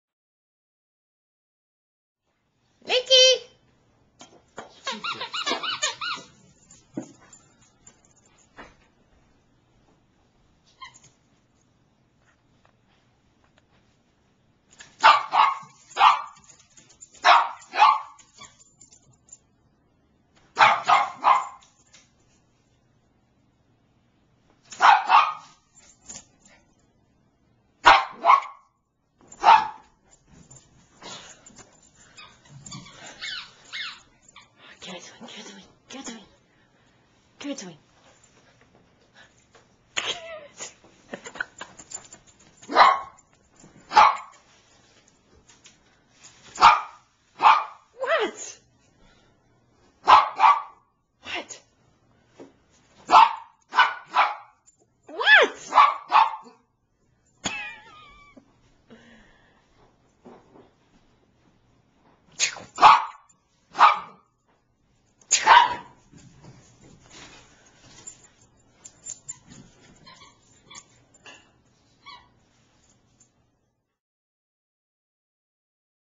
Niqqi Wants Her Squeaky
Niqqi, the Bichon Frise, loves to play with all her toys...but she really likes her "squeaky!" She begs her mom to throw her squeaky toy by barking "Give me my Squeaky", and runs after it when, finally, her mom does.  Niqqi tweets on twitter